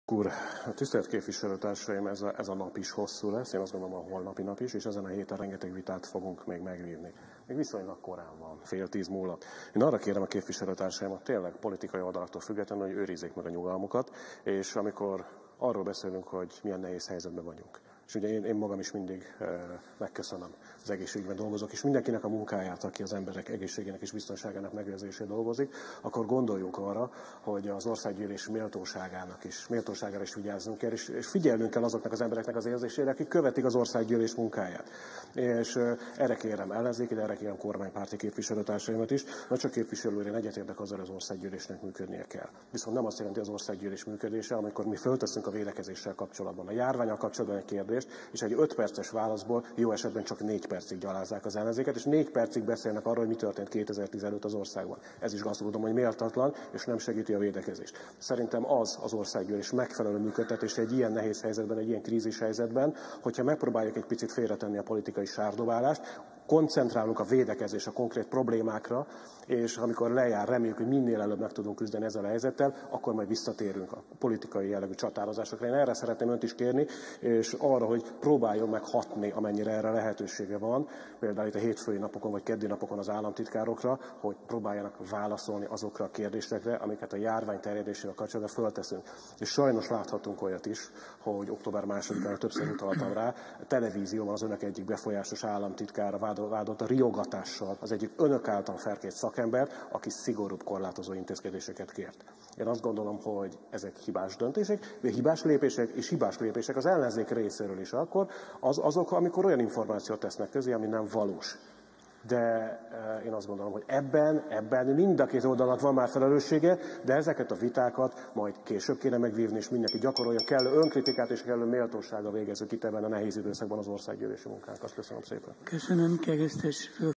Az Országgyűlés méltóságáról
Keresztes L. Lóránt kétperces felszólalása a Veszélyes Áruk Nemzetközi Közúti Szállításáról szóló Európai Megállapodás kihirdetéséről szóló 1979. évi 19. törvényerejű rendelet hatályon kívül helyezéséről szóló törvényjavaslat vitájában.